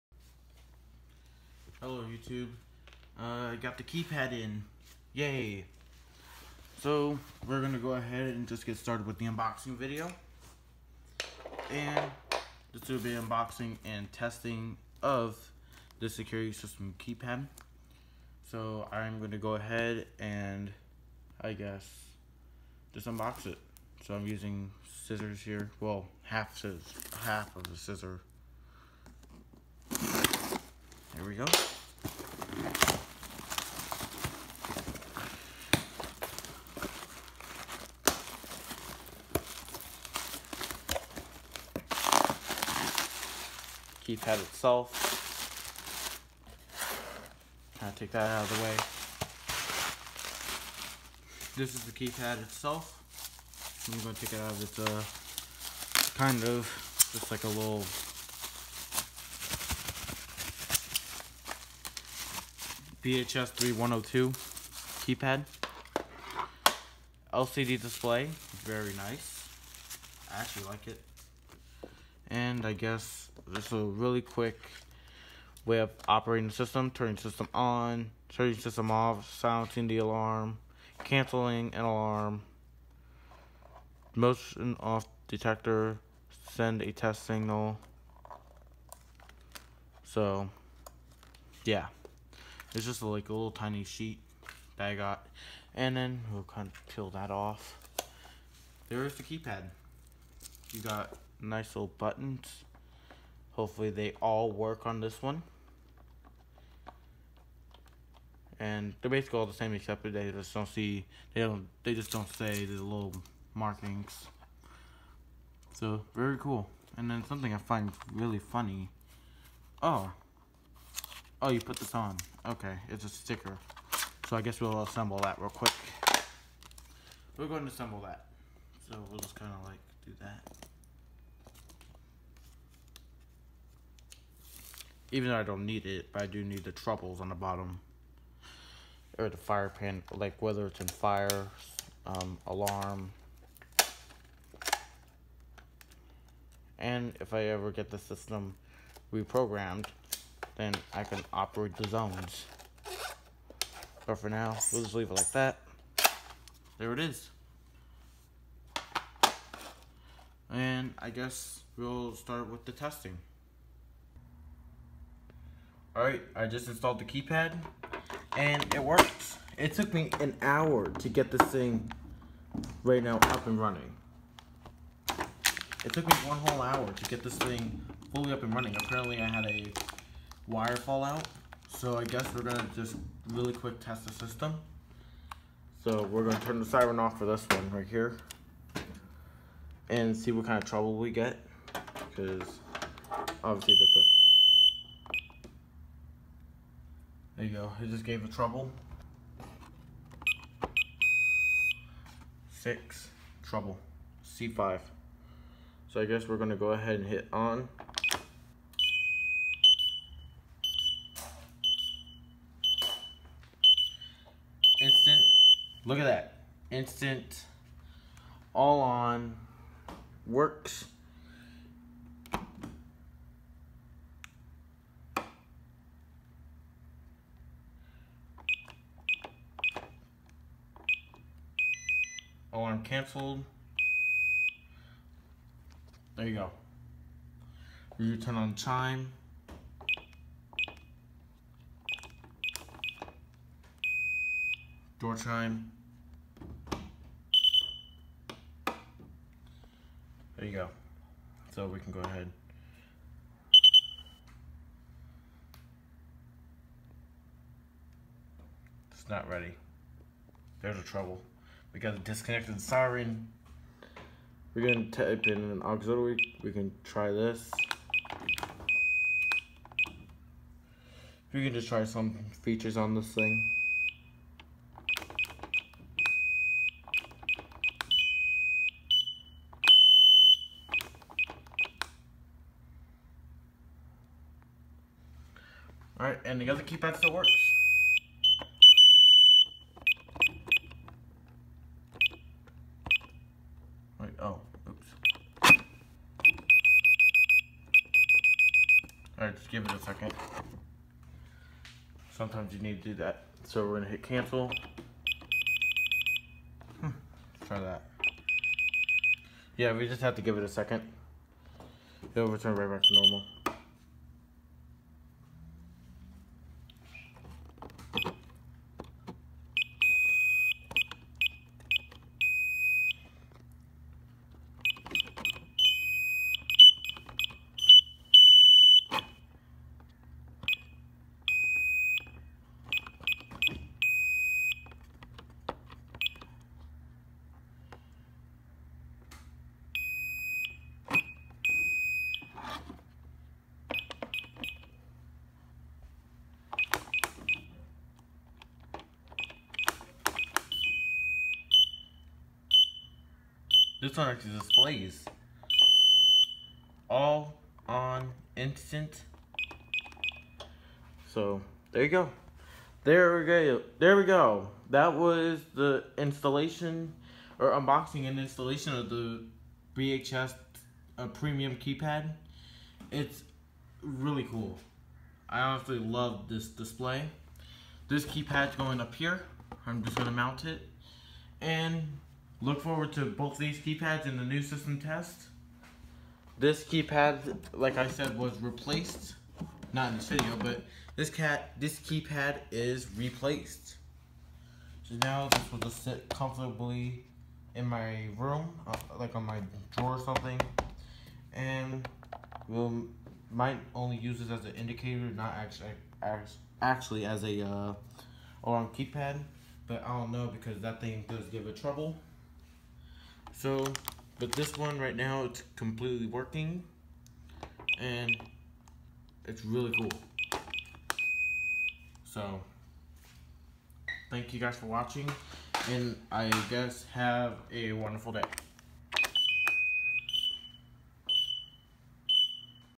Unboxing and installation of the BHS 3102 premium security keypad.
This video I will be unboxing a keypad I have waited for a week. Led display and simple programming. Sadly the keypad took a hour to see what was wrong with it. A wire fell out so it is up and running.